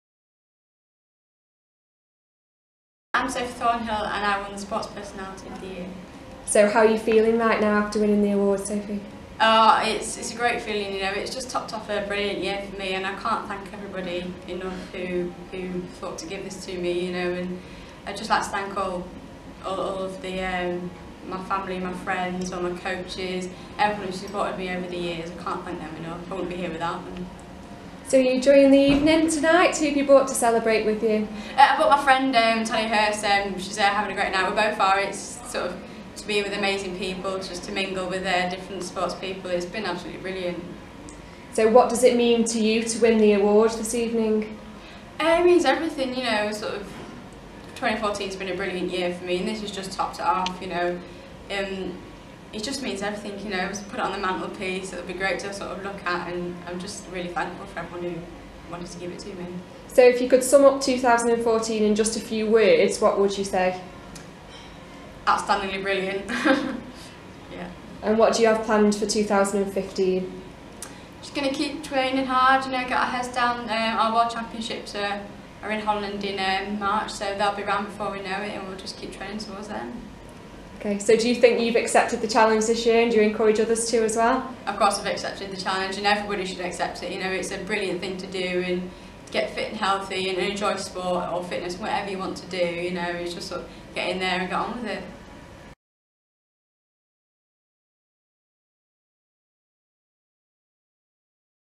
Sophie Thornhill Sports Personality of the Year
Everybody Awards welcomes Sophie Thornhill and awards her with Sports Personality of the Year Award 2014 Cheshire East